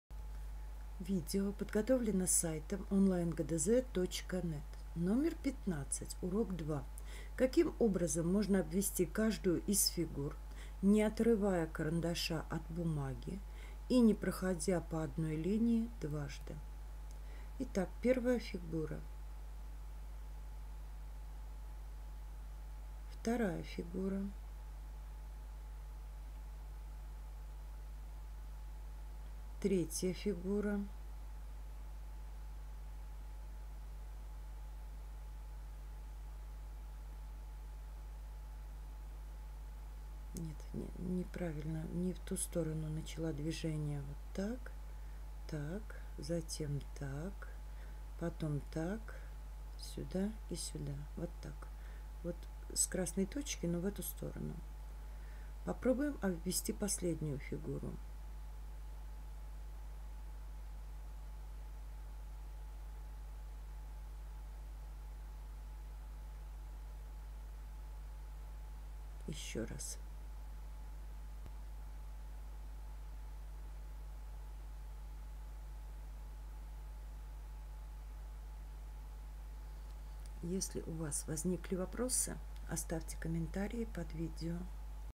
Урок 2 Задание 15 – ГДЗ по математике 2 класс (Петерсон Л.Г.) Часть 2
В данном номере определяем, каким образом можно обвести каждую из фигур, не отрывая карандаша от бумаги и не проходя по одной линии дважды.